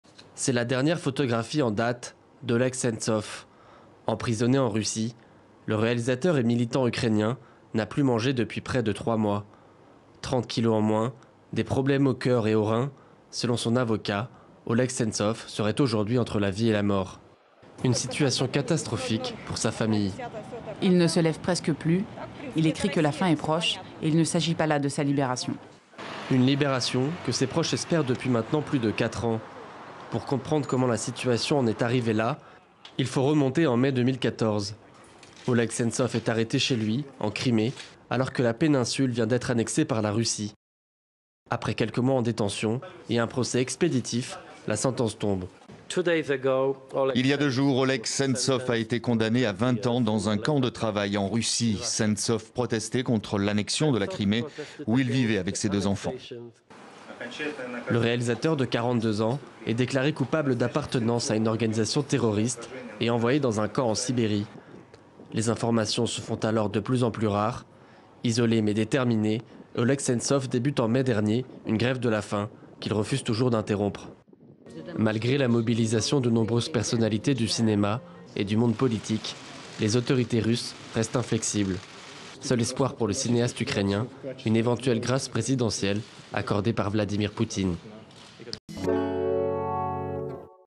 En Russie, Oleg Sentsov, réalisateur et prisonnier politique
🇷🇺 Russie - Le cinéaste ukrainien Oleg Sentsov, opposé à l'annexion de la Crimée, est détenu en Russie depuis 2014. Il a entamé une grève de la faim depuis 3 mois. Il serait en très mauvaise santé